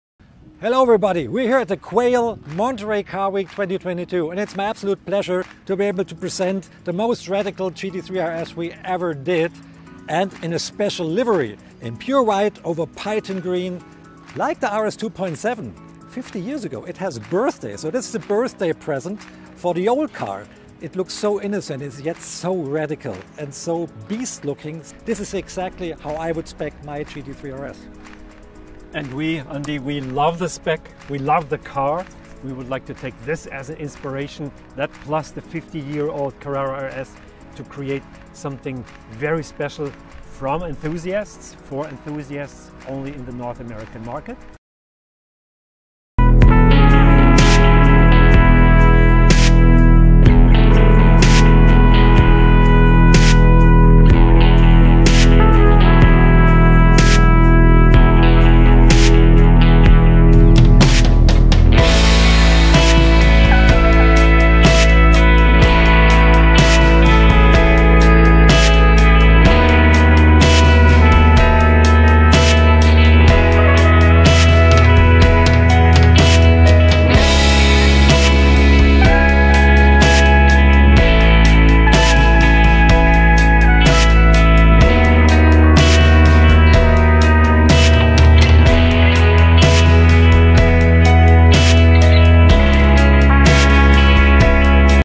The Porsche 911 GT3 RS Carrera RS 2 7 Tribute
Appropriately for Monterey Car Week, the model on display is extensively personalized. Finished in white with key areas in Python Green, the car is a direct reference to the iconic white and green color configuration offered in 1972 on the first 911 “RS” model as well as a new interpretation of it. In this instance, the body is finished in non-metallic white including side air intakes painted to match with model designation graphics on the sides and rear in Python Green, forged aluminum wheels in Python Green with white pinstripes. Exclusive Design taillights emphasize the green-on-white theme by eliminating the red color from the rear taillights and third brake light. Finally, the side plates on the left and right ends of the large rear wing are painted white with RS logos in Python Green.

“Porsche Exclusive Manufaktur allows customers to personalize their car to a point where the vehicle is as much a reflection of them as it is of Porsche,” said Karl-Heinz Volz, Director of Exclusive Manufaktur Options. “Personalization is a Porsche tradition dating back to our first production model, the 356, when a customer requested a wiper be added to the rear window of their car. Today it is an incredibly extensive personalization program, offering options ranging from small changes to true one-off creations.”